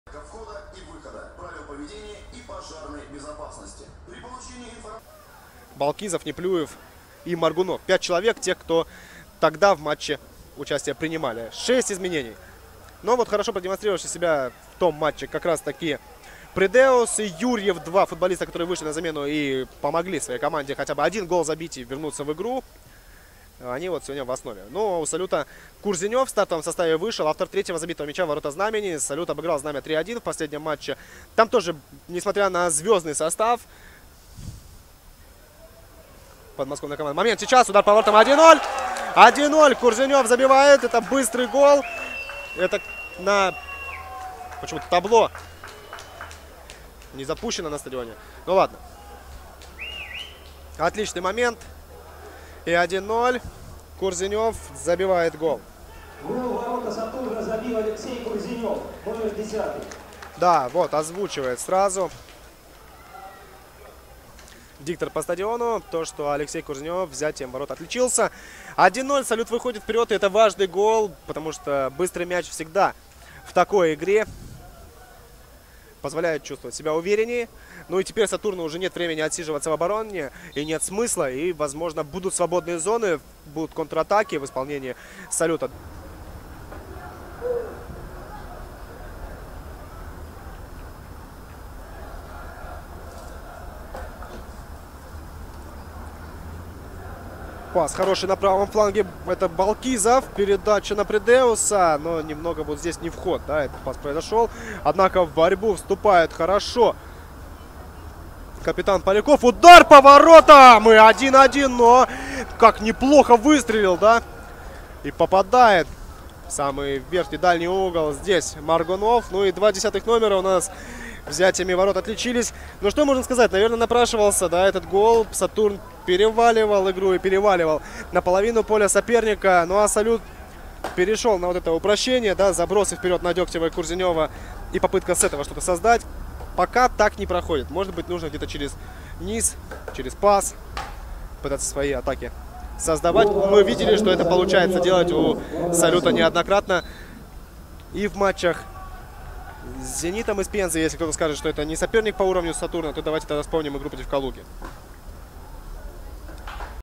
«Салют» Белгород - «Сатурн» Раменское 1:1. Голы матча
«Салют» - «Сатурн» 1:1 (1:1)

Голы: Курзенёв, 5 (1:0); Моргунов, 34 (1:1).

Вторая лига, группа 3.2, 16-й тур.
15 октября 2022 г.
г. Белгород, стадион «Салют».
300 зрителей.